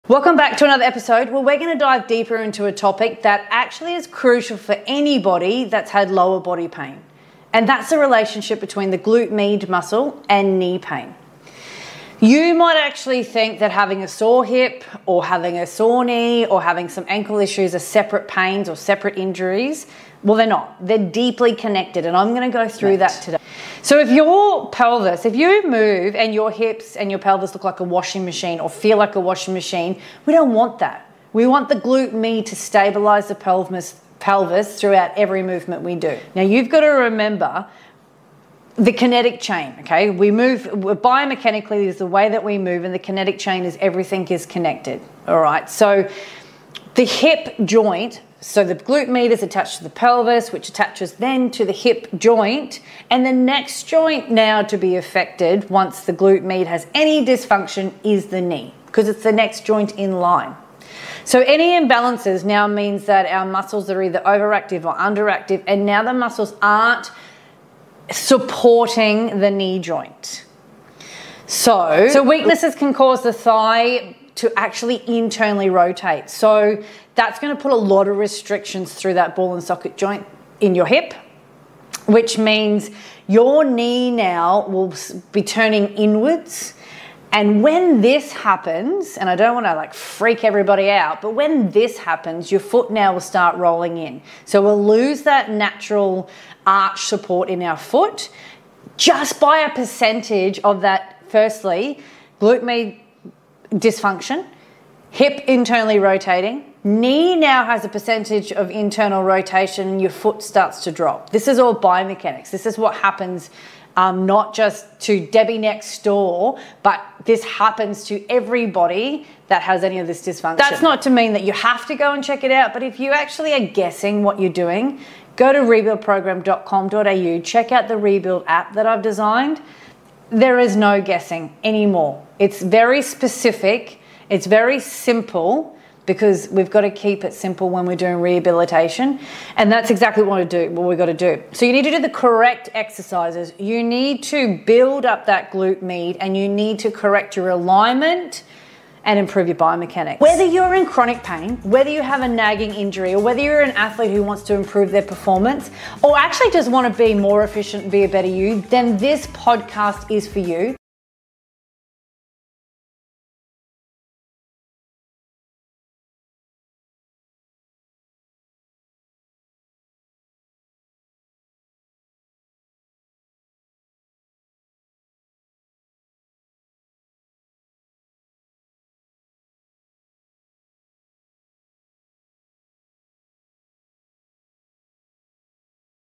➡️Understanding Your Knee Pain and FIXING IT!⬅️
This powerhouse muscle in our hips plays a pivotal role in stabilizing our pelvis and maintaining lower body alignment, yet it frequently gets neglected in standard knee pain treatment protocols.